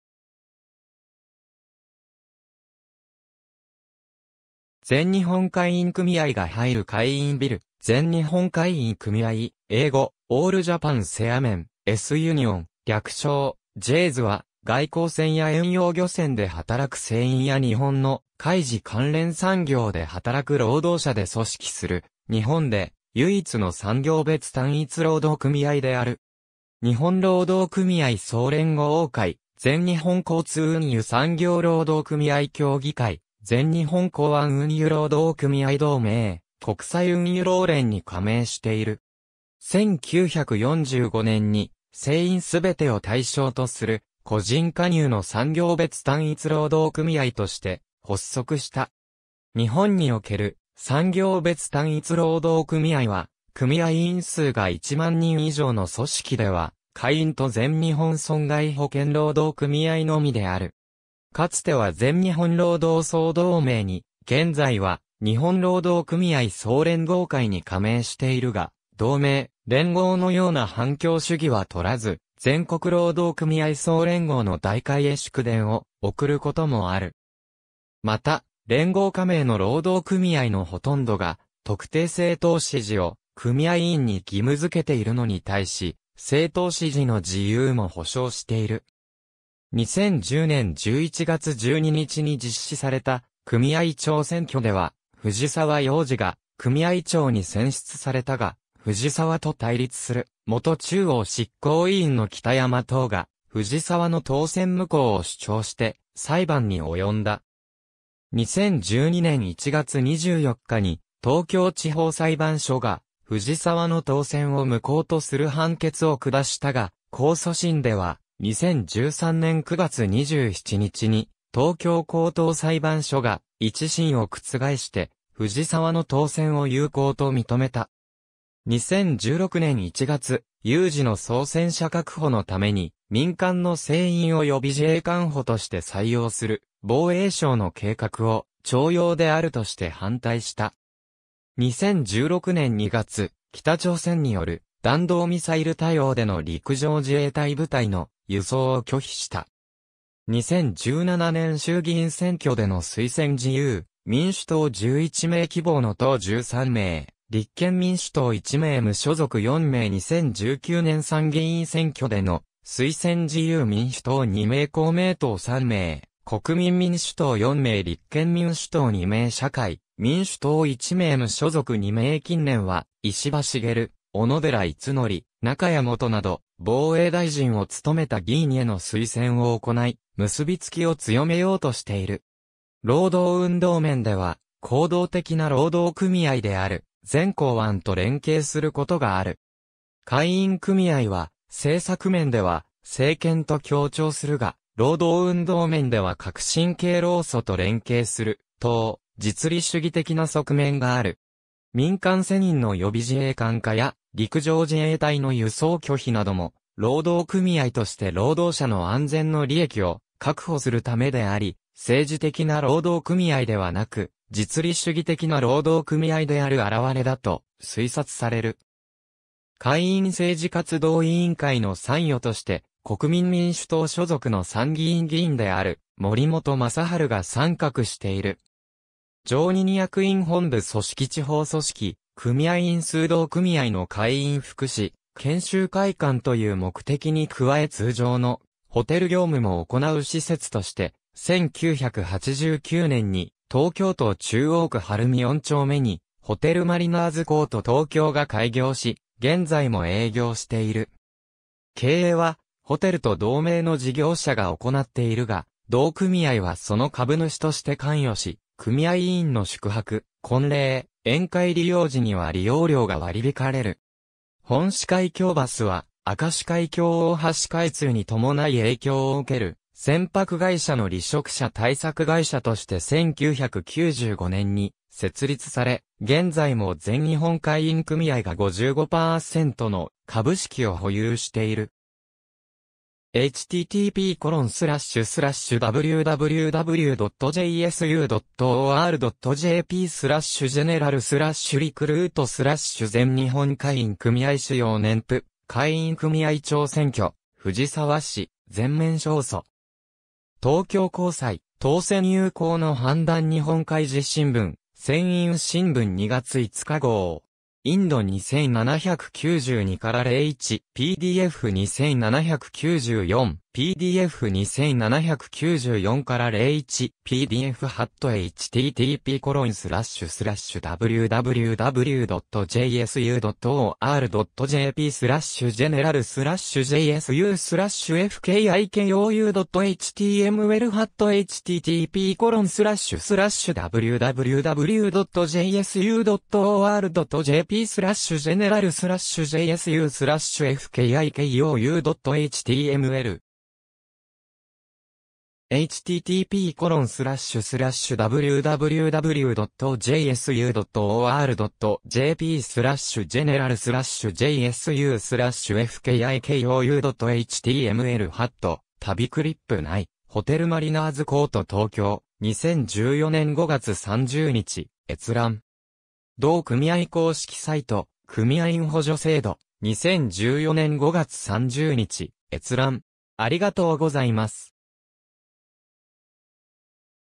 全日本海員組合
全日本海員組合が入る海員ビル（六本木） 全日本海員組合（ぜんにほんかいいんくみあい、略称：海員（かいいん）、英語：All Japan Seamen's Union、略称：JSU）は、外航船や遠洋漁船で働く船員や日本の海事関連産業で働く労働者で組織する日本で唯一の産業別単一労働組合である。
日本労働組合総連合会（連合）、全日本交通運輸産業労働組合協議会（交運労協）、全日本港湾運輸労働組合同盟（港運同盟）、国際運輸労連（ITF）に加盟している。
1945年（昭和20年）に、船員すべてを対象とする個人加入の産業別単一労働組合として発足した。
日本における産業別単一労働組合は組合員数が1万人以上の組織では海員と全日本損害保険労働組合（全損保）のみである。
かつては全日本労働総同盟（同盟）に、現在は日本労働組合総連合会（連合）に加盟しているが、同盟・連合のような反共主義はとらず、全国労働組合総連合（全労連）の大会へ祝電を送ることもある。
また、連合加盟の労働組合のほとんどが特定政党支持を組合員に義務付けているのに対し、政党支持の自由も保障している（1960年11月に民社党支持を大会決定したが、1973年11月に特定政党支持の廃止を大会決定した）。
2010年（平成22年）11月12日に実施された組合長選挙では藤澤洋二が組合長に選出されたが、藤澤と対立する元中央執行委員の北山等が藤澤の当選無効を主張して裁判に及んだ。
2012年（平成24年）1月24日に東京地方裁判所が、藤澤の当選を無効とする判決を下したが、控訴審では、2013年（平成25年）9月27日に東京高等裁判所が、一審を覆して藤澤の当選を有効と認めた。
2016年1月、有事の操船者確保のために民間の船員を予備自衛官補として採用する防衛省の計画を、徴用であるとして反対した。
2016年2月、北朝鮮による弾道ミサイル対応での陸上自衛隊部隊の輸送を拒否した。
2017年衆議院選挙での推薦（党派別） 自由民主党　11名 希望の党　　13名 立憲民主党　1名 無所属　　　4名 2019年参議院選挙での推薦（党派別） 自由民主党　2名 公明党　　　3名 国民民主党　4名 立憲民主党　2名 社会民主党　1名 無所属　　　2名 近年は石破茂、小野寺五典、中谷元など防衛大臣を務めた議員への推薦を行い結びつきを強めようとしている。
労働運動面では行動的な労働組合である全港湾と連携することがある。
海員組合は政策面では政権と協調するが、労働運動面では革新系労組と連携する等、実利主義的な側面がある。
民間船員の予備自衛官化や陸上自衛隊の輸送拒否なども労働組合として労働者の安全の利益を確保するためであり、政治的な労働組合ではなく実利主義的な労働組合である現れだと推察される。
海員政治活動委員会の参与として国民民主党所属の参議院議員である森本真治が参画している。
常任役員 本部組織 地方組織 組合員数 同組合の海員福祉研修会館という目的に加え通常のホテル業務も行う施設として、1989年に東京都中央区晴海4丁目に「ホテルマリナーズコート東京」が開業し、現在も営業している。
経営はホテルと同名の事業者が行っているが、同組合はその株主として関与し、組合員の宿泊・婚礼・宴会利用時には利用料が割り引かれる。
本四海峡バスは、明石海峡大橋開通に伴い影響を受ける船舶会社の離職者対策会社として1995年に設立され、現在も全日本海員組合が55パーセントの株式を保有している。
^ 全日本海員組合 主要年譜 ^ 海員組合長選挙/藤澤氏「全面勝訴」。
東京高裁、当選有効の判断日本海事新聞 ^ 船員しんぶん2月5日号.indd - 2792-01.pdf ^ 2794.pdf - 2794-01.pdf ^